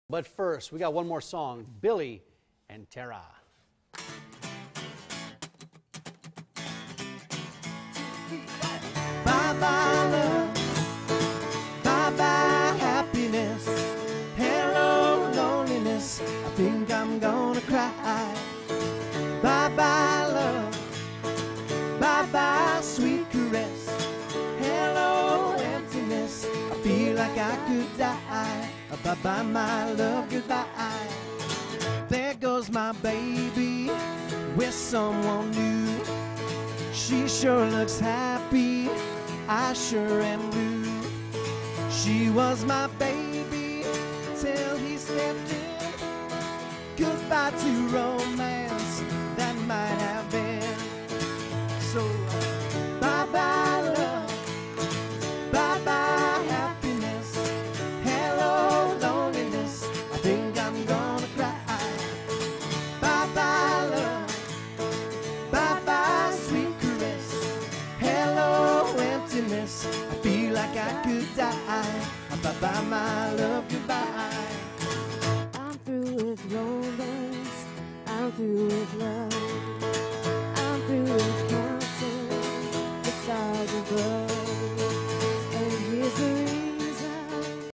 Billy McGuigan and Tara Vaughn
Billy McGuigan and Tara Vaughn have a live performance to close the show